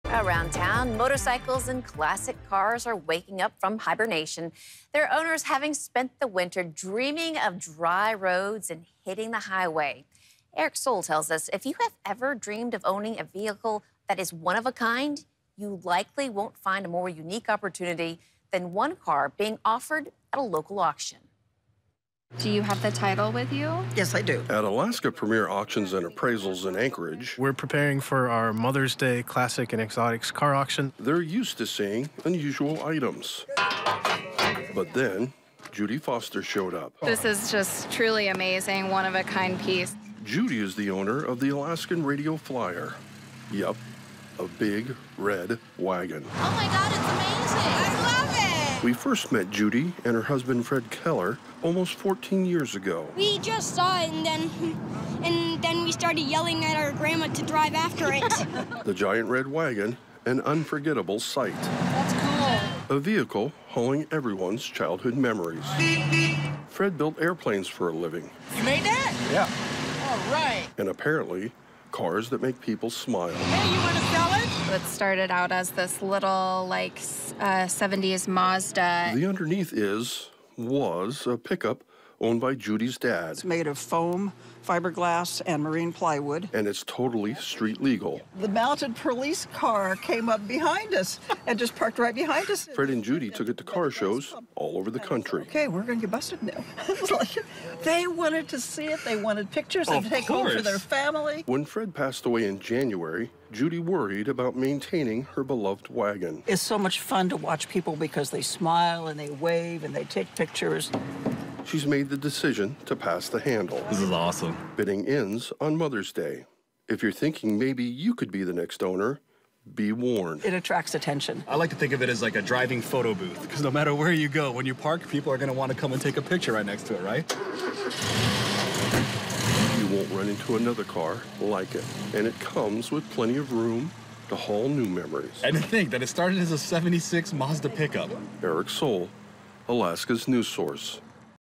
Much-loved custom vehicle will soon be headed to new home
There’s a car currently up for auction in Anchorage that almost everyone has a childhood connection to. It’s not a station wagon — but it is a wagon.